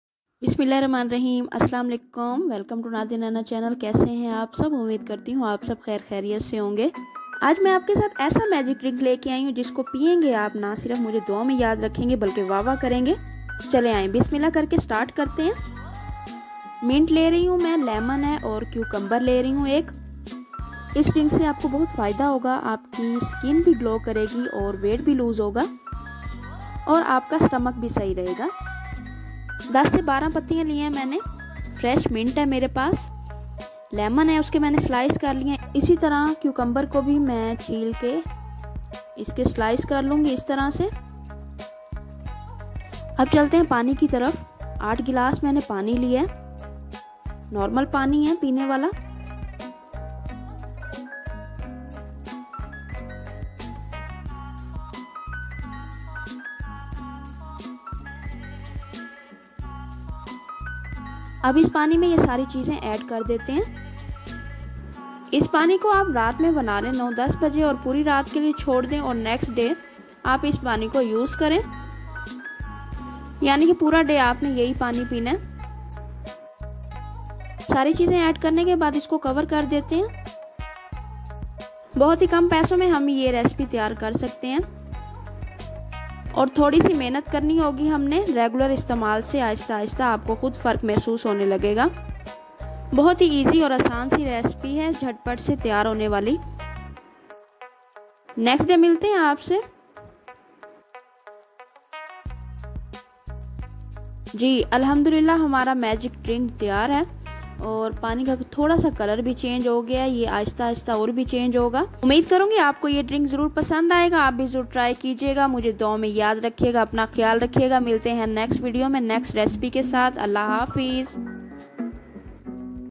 Diet Cucumber Magic Water | Cucumber Detox For Lose Weight
Detox Water for weight loss and clear skin | Cucumber Water |

Ingredients

1. Cucumber
2. Mint
3. Lemon
4. Water

Best Wishes!